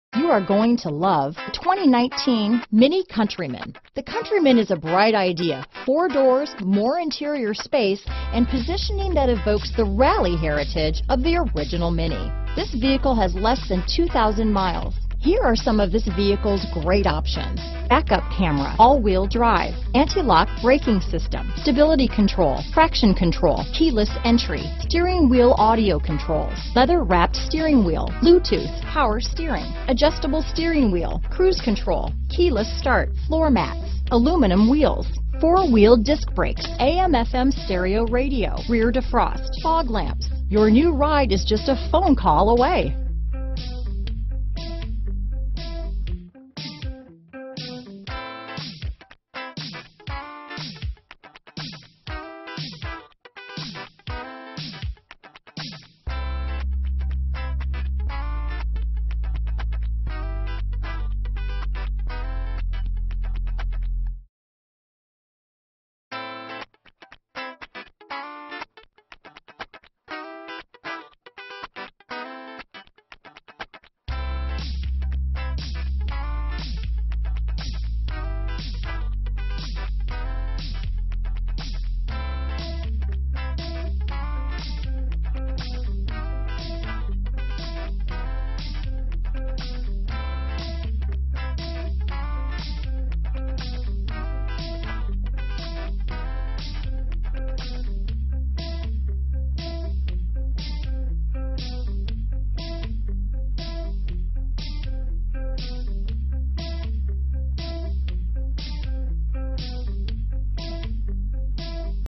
2019 MINI Countryman Fletcher, Hendersonville, Waynesville, Marion, Asheville, FL D2659
Thunder Gray Metallic Used 2019 MINI Countryman available in Asheville, Florida at BMW of Asheville. Servicing the Fletcher, Hendersonville, Waynesville, Marion, FL area.
2019 MINI Countryman Cooper S - Stock#: D2659 - VIN#: WMZYT5C54K3E62659

GREAT MILES 1,362! FUEL EFFICIENT 30 MPG Hwy/21 MPG City!